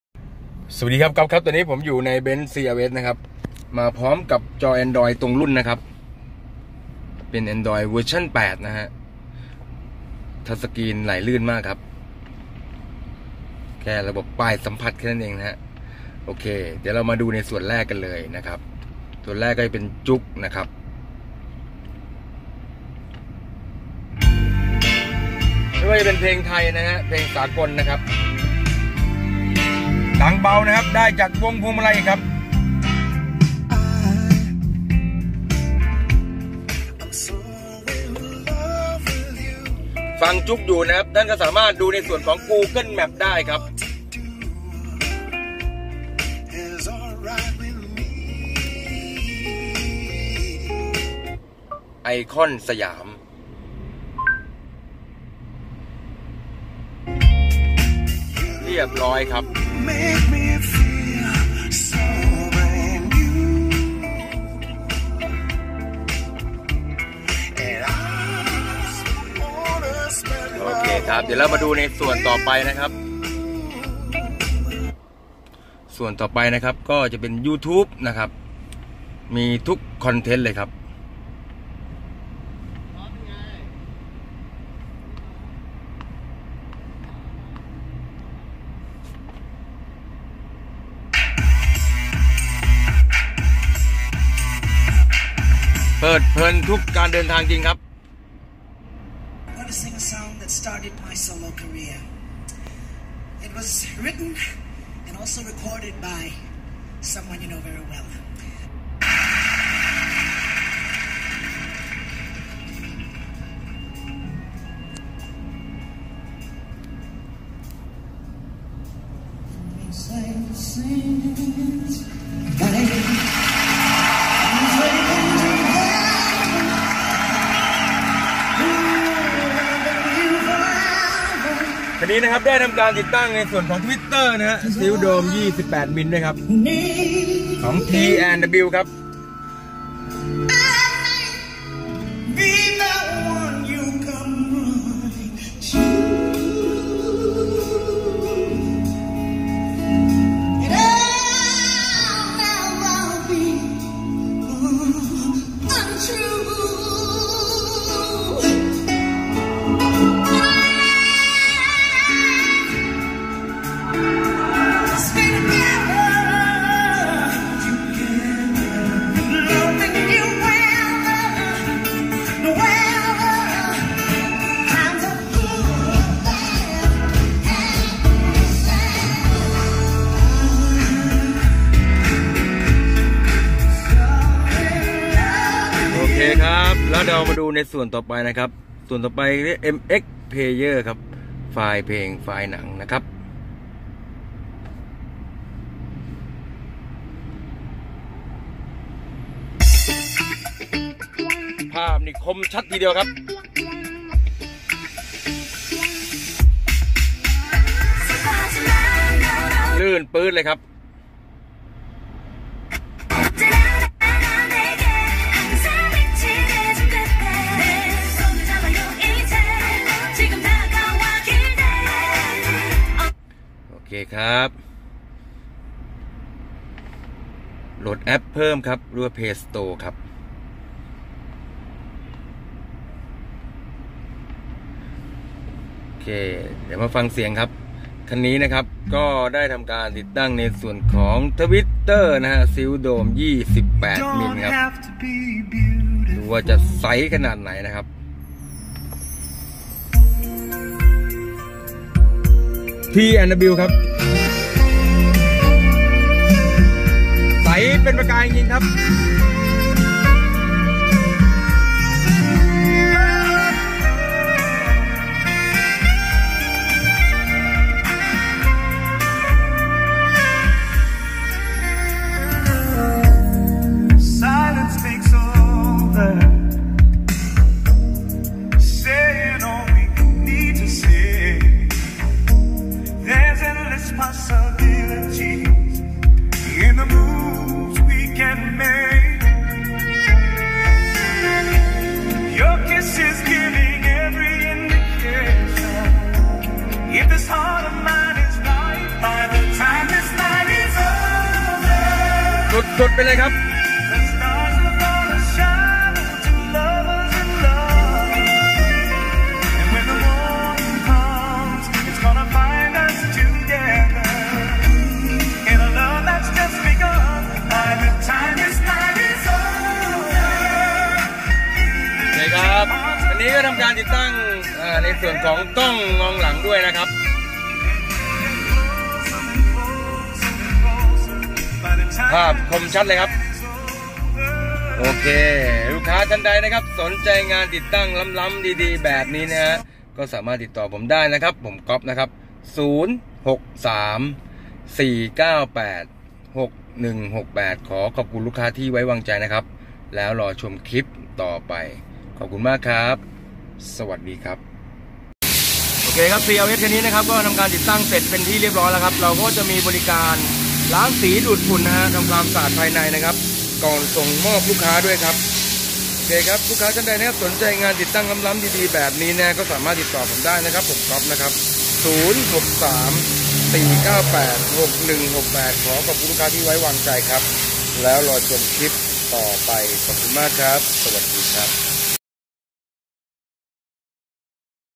BENZ CLS W219 กับจอวิเศษ ANDROID ตรงรุ่นมาพร้อมกล้องมองหลัง
ชุดติดตั้งจอ ANDROID ตรงรุ่น สามารถเล่นไฟล์เพลง.หนัง JOOX YOUTUBE GOOGLE MAPS. MX PLAYER
ความบันเทิงครบคัน
เพิ่มความปลอดภัยด้วยกล้องมองหลัง
เสริมด้วยเสียงใสเป็นประกายด้วย
TWEETER T&W
ท่านใดสนใจงานติดตั้งสอบถามได้ที่